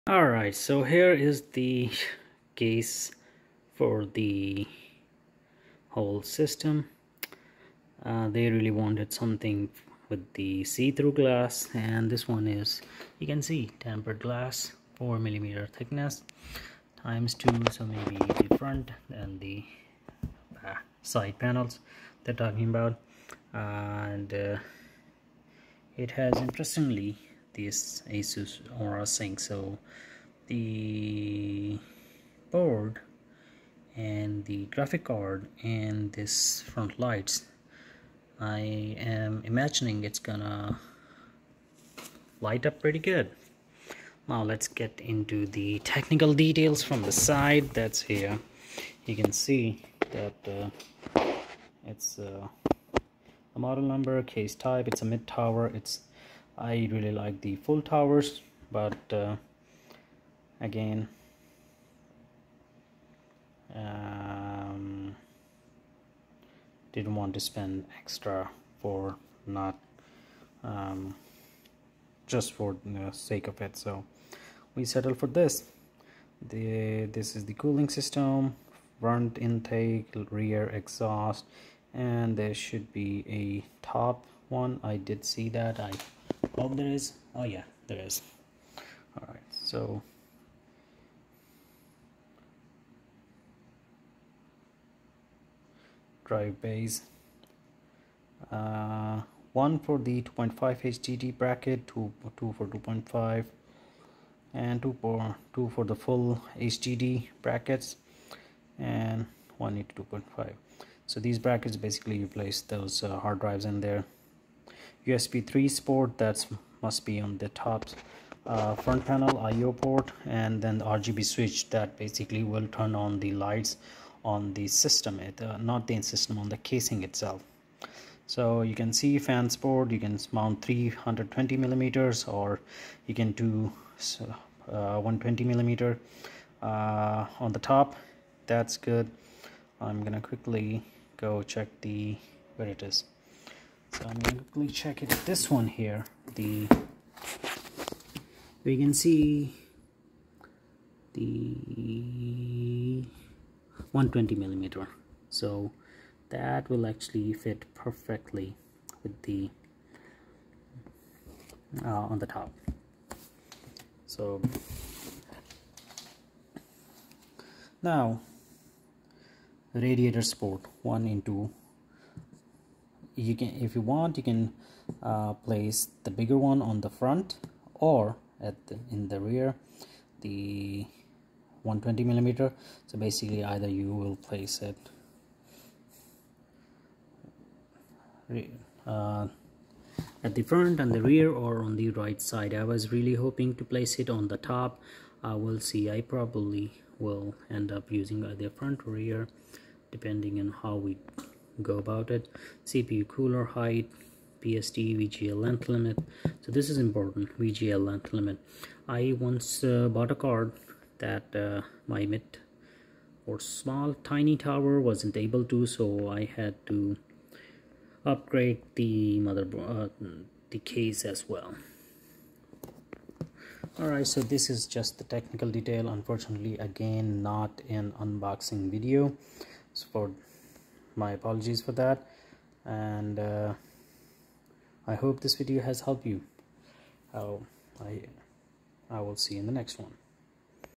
Not Unboxing ~ Thermal Take Leval 20MT ARGB PC case
so we are using this TT ThermalTake case to house our build